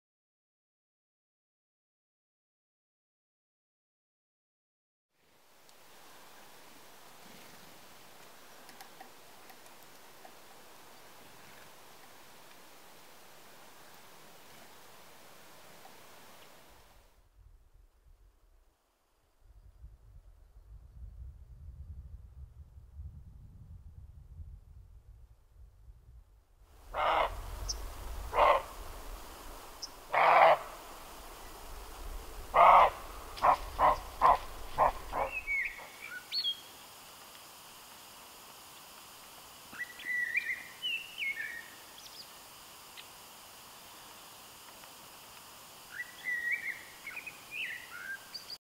Fredagkväll
Vad jag såg under en kort kvällspromenad (mellan kl 18 och 19) i gårdens omedelbara närhet...